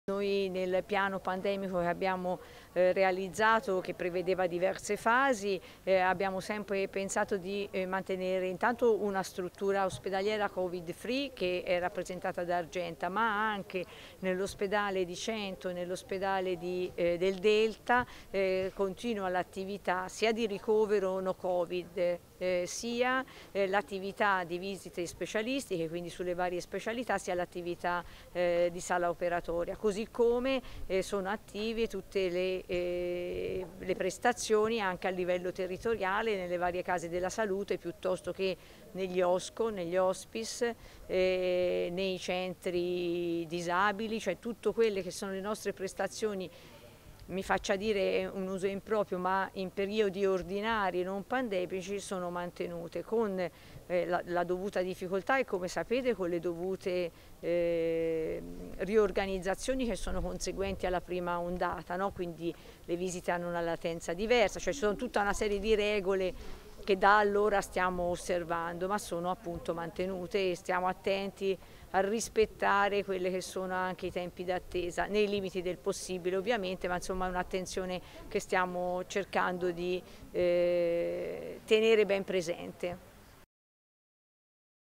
Usl Ferrara: "Al momento no rinvii prestazioni come in primavera"
Questa mattina a Cento c'è stata un'importante presentazione di una donazione all'ospedale Santissima Annunziata che potenzierà l'assistenza domiciliare, strategia sanitaria fondamentale in questa fase della lotta al covid.

Ma nel corso della conferenza è intervenuta anche la direttrice generale dell'azienda Usl di Ferrara, Monica Calamai. Azienda sanitaria che gestisce tra l'altro tutti gli ospedali ferraresi a parte Cona. Strutture in provincia e in città per cui al momento, in questa seconda ondata, non sono previsti rinvii di prestazioni sanitarie programmate come avvenne la scorsa primavera.